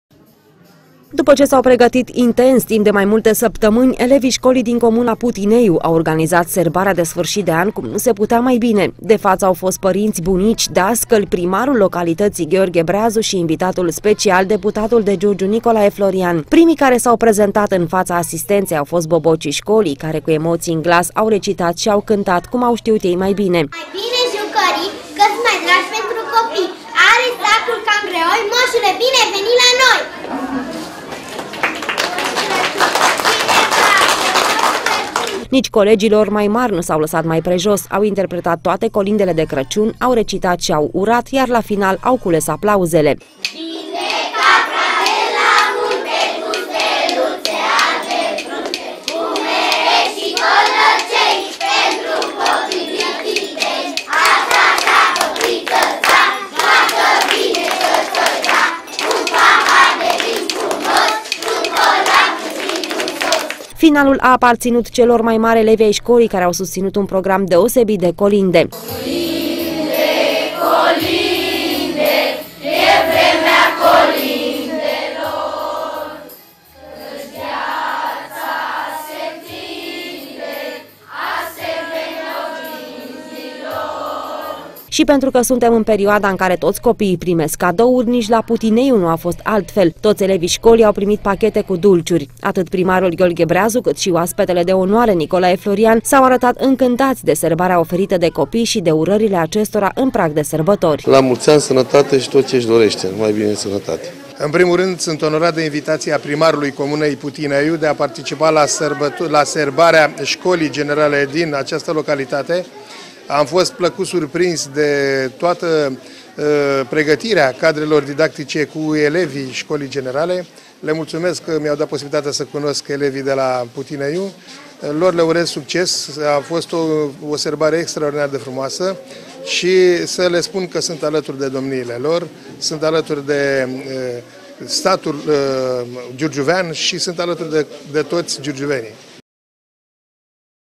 SI LA PUTINEIU VIN SARBATORILE
Sfârșit de an cu poezii, cântece și colinde la școala din comuna Putineiu. Elevii școlii din localitate au pregătit o serbare ca la carte, mai ales că au avut un invitat deosebit... deputatul de Giurgiu, Nicolae Florian. Mai mici sau mai mari, școlarii din Putineiu s-au prezentat impecabil, semn că tradițiile sunt respectate și perpetuate din generație în generație.